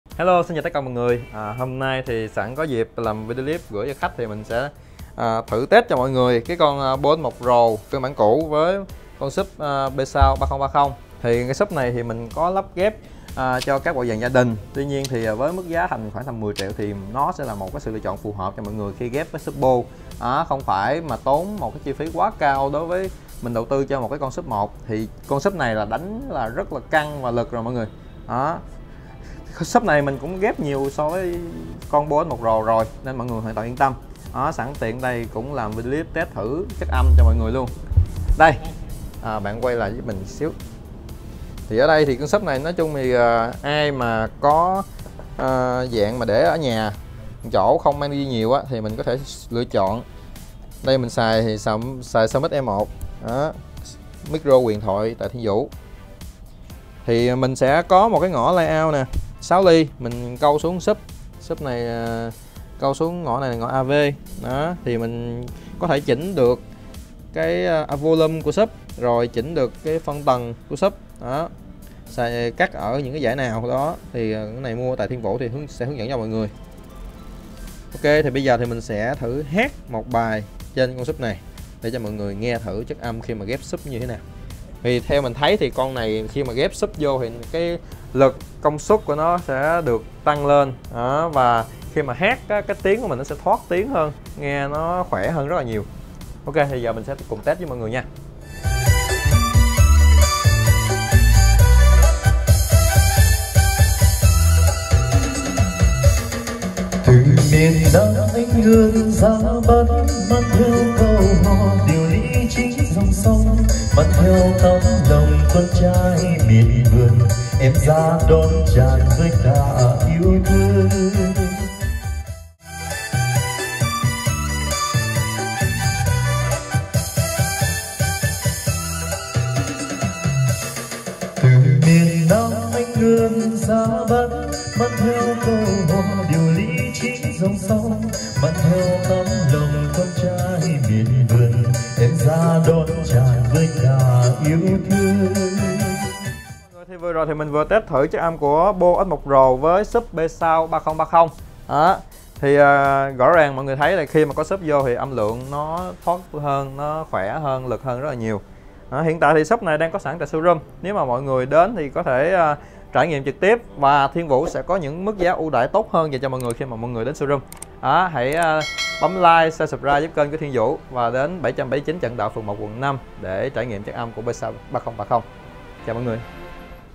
Trải Nghiệm Chất Âm Loa Sub Chuyên Ghép Với BOSE S1 PRO | P'Sound Sub 3030A | Thiên Vũ Audio
P'Sound Sub 3030A mức công suất 600W giúp âm thanh vang to, mạnh mẽ bao trùm toàn bộ không gian. Độ sâu dải trầm xuống đến 24Hz tạo nên những nốt âm trầm ấm, uy lực.

🔻 𝐈𝐧𝐟𝐨𝐫𝐦𝐚𝐭𝐢𝐨𝐧 🔻
🔻 𝗙𝗼𝗹𝗹𝗹𝗼𝘄 𝗠𝗲 🔻
®️ Nội dung khiếu nại bản quyền:
0:00 Mở đầu
1:00 Cách kết nối
1:25 Điểm lợi
1:40 Test có sub
2:05 Test không sub
2:30 Tổng kết